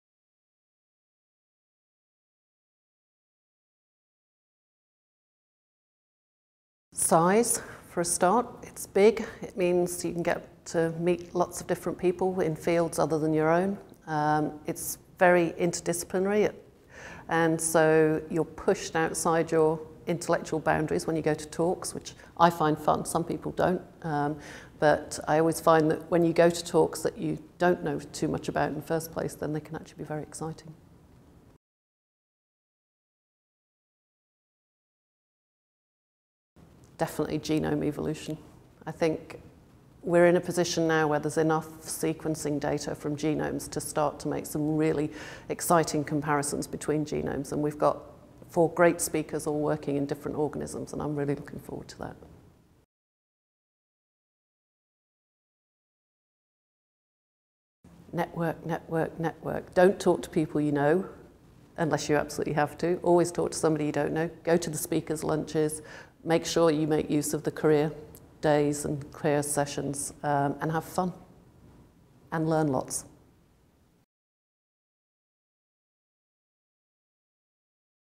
Interview with Jane Langdale, conference chair The EMBO Meeting 2011
Jane Langdale, Co-chair of The EMBO Meeting 2011 and Professor of Plant Development at Oxford University. Jane highlights aspects of the programme; gives advice to students participating in the conference; and talks about what makes this conference so unique.